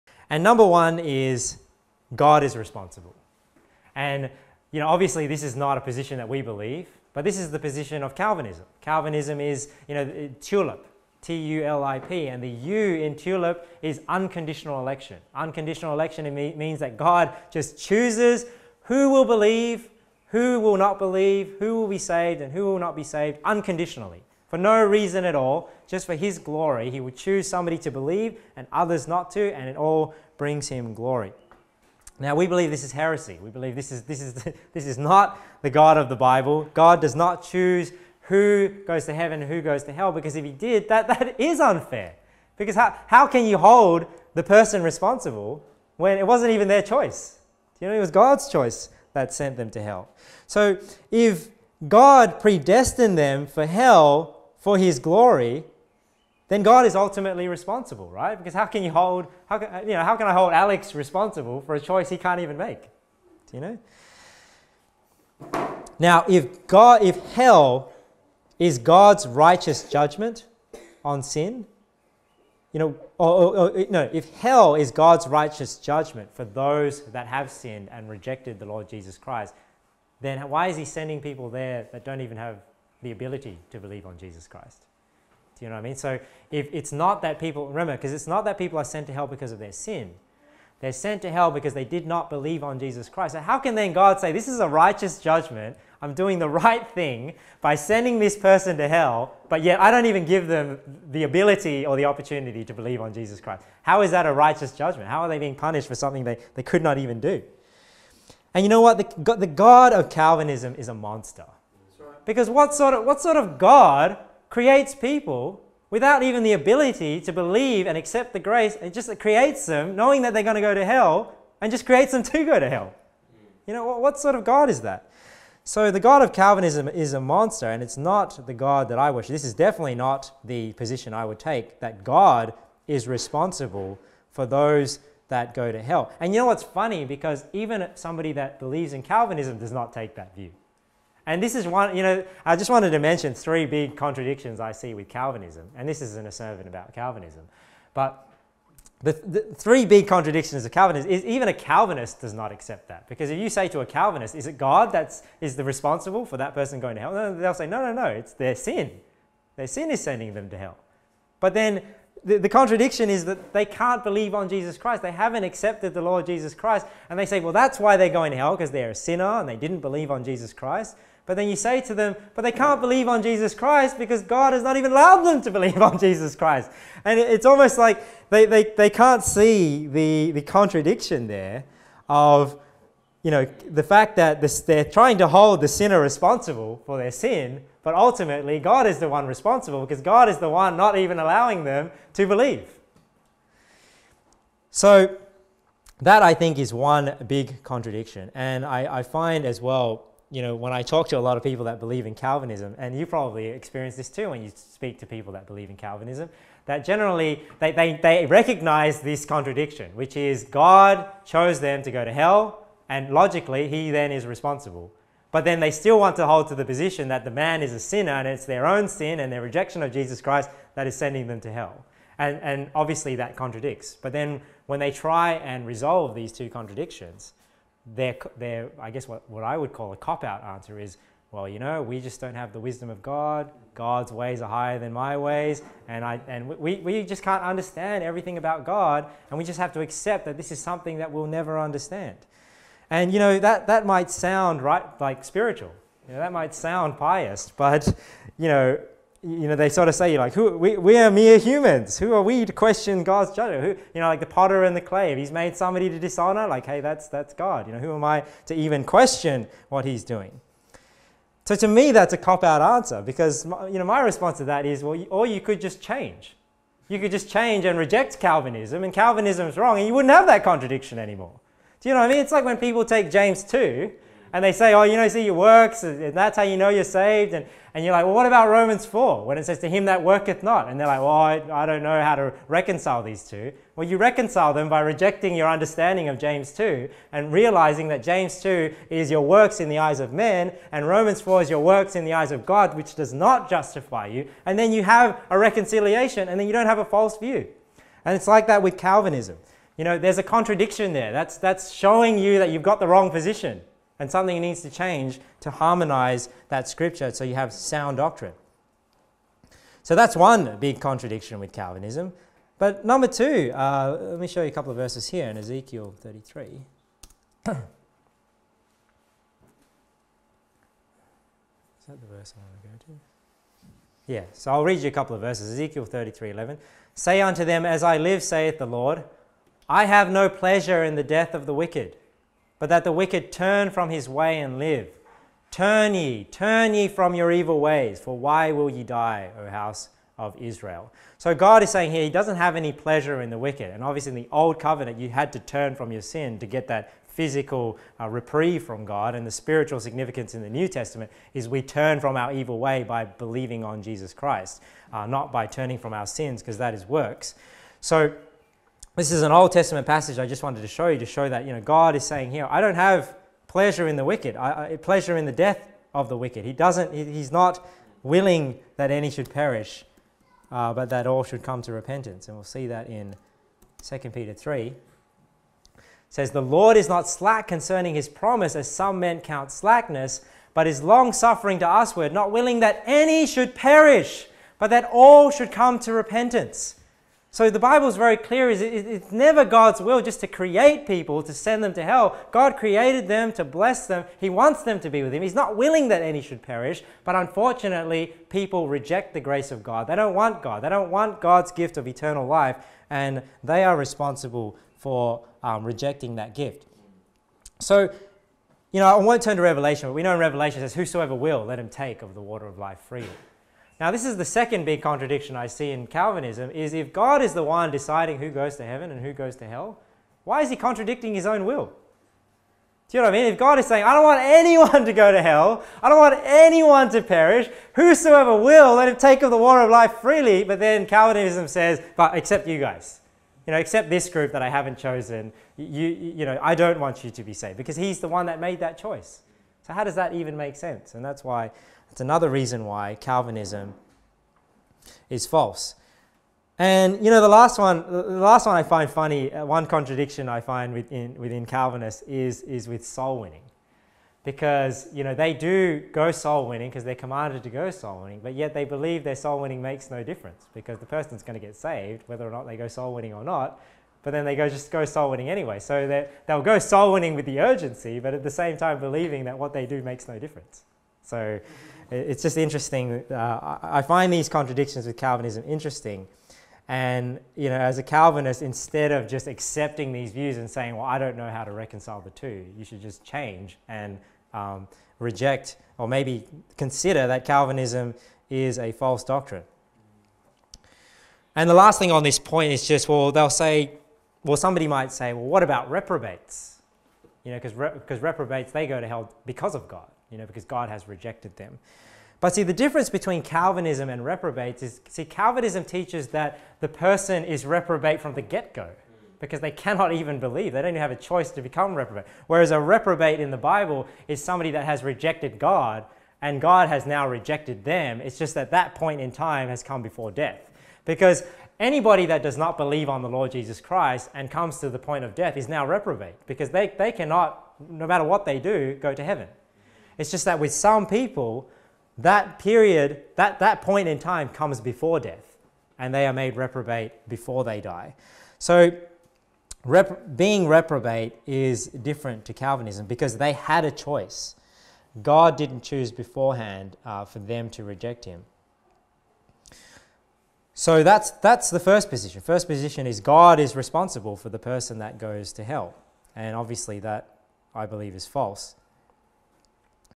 Is God Ultimately Responsible for Why a Soul Goes to Hell? (3 Contradictions in Calvinism)
This is a clip from the sermon entitled "The Soul That Has Never Heard The Gospel" preached by Victor Tey at The Church in Punchbowl on Sunday, 25th October 2015.